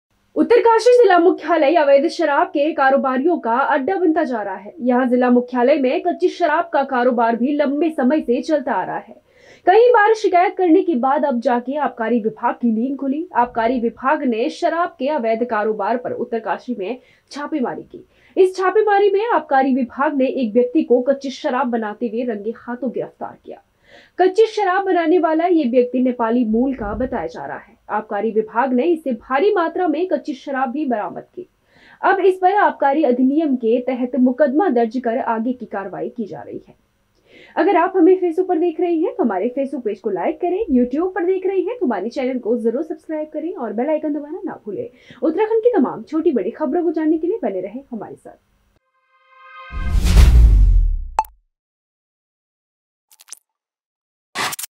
जिला मुख्यालय में ही बना दिया अवैध शराब कारोबार का अड्डा
Uttarakhand Legislative Assembly election 2022